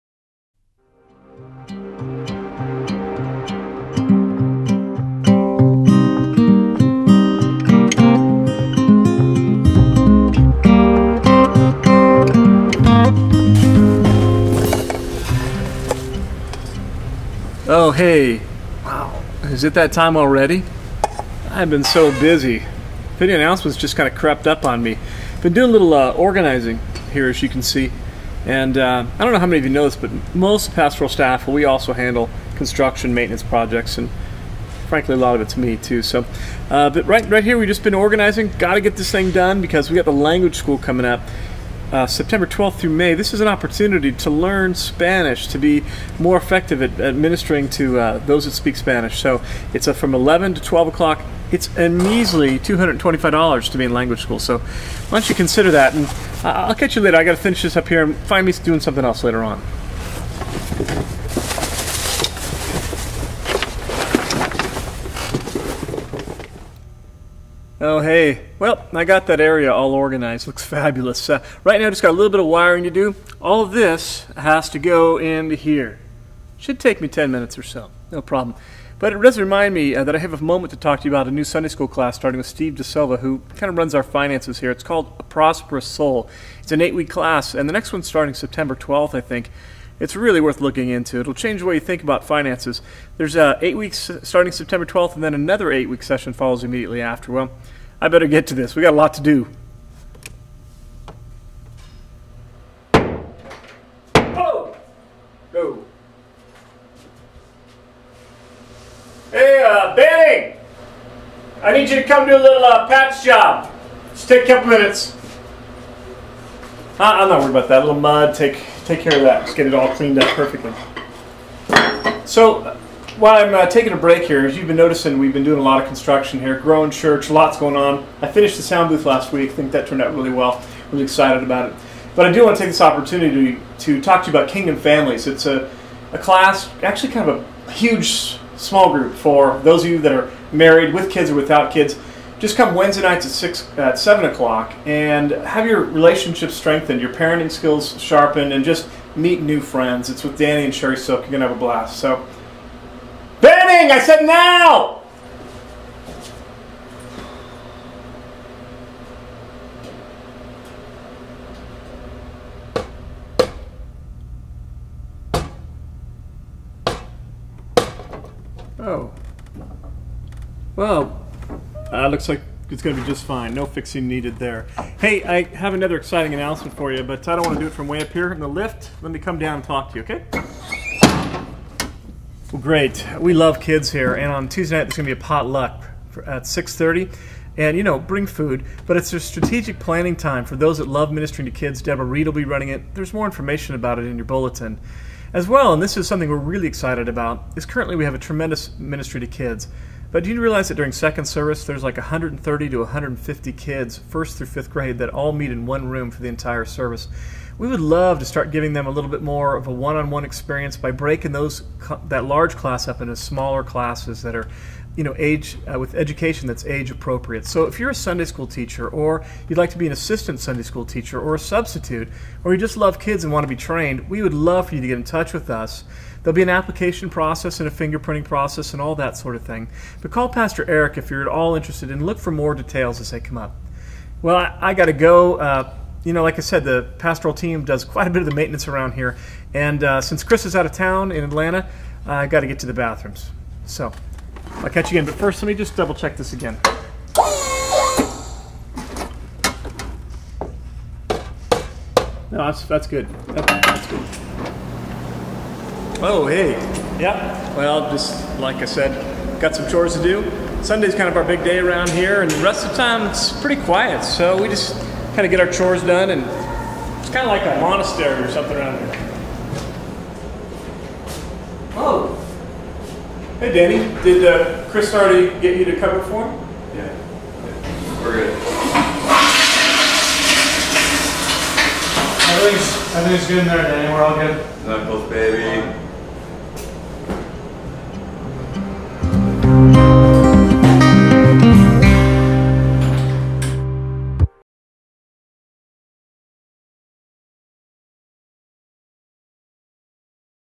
Worker Bees - 08.20.06
The weekly Video Announcements video from Bethel Church for August 20, 2006.